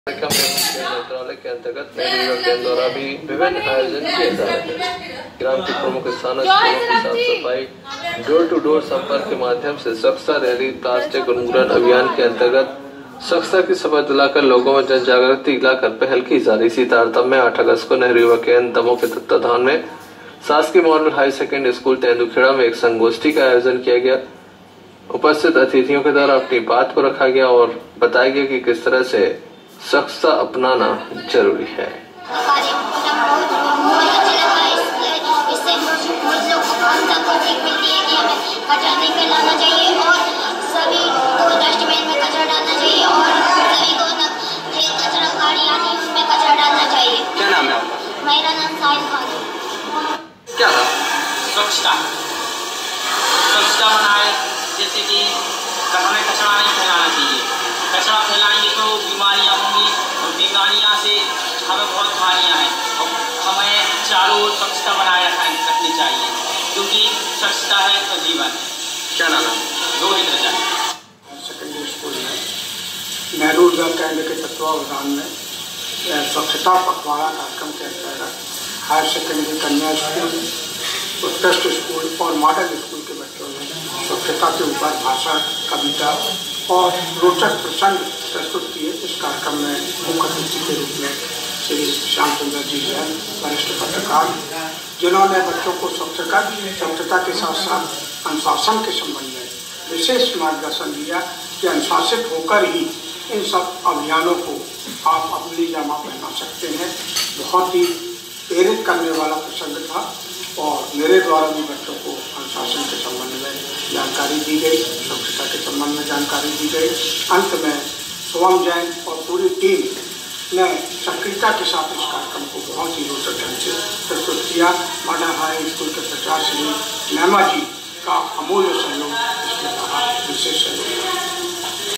तेंदूखेड़ा-/नेहरू युवा केन्द्र की और से स्वच्छता पखवाड़ा उत्सव की एक अनोखी पहल,,,
अर्पित बड़कुल